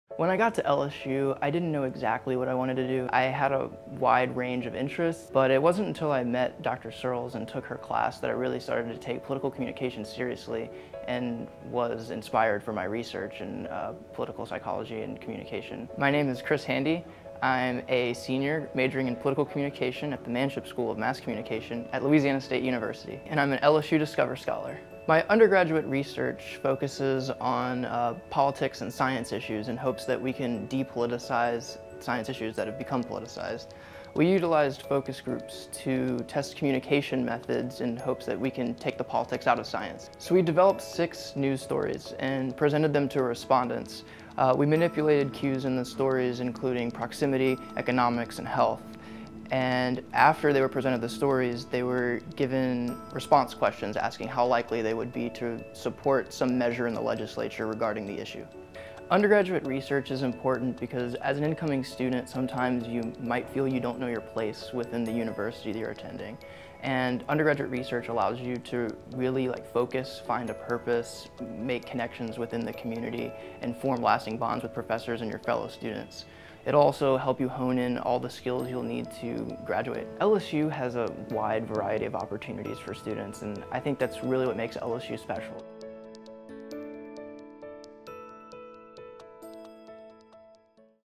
LSU Discover Scholar - Chris Handy
LSU Discover Scholars are those students who exemplify outstanding undergraduate research or creative endeavors within their fields. Every year a group of distinguished undergraduate students are selected from the senior colleges for excellence in their research or creative achievements.

Chris Handy is an Ogden Honors College Senior majoring in Political Communication. Chris’s project is titled, Depoliticizing Science: The Effects of Local Crises on Partisan Science Policy. Research by Yale Law Professor Dan Kahan inspired his idea for this project.  Kahan’s work on the Cultural Cognition Project and, in particular, political psychology and science communication, motivated Chris to approach this new field of research.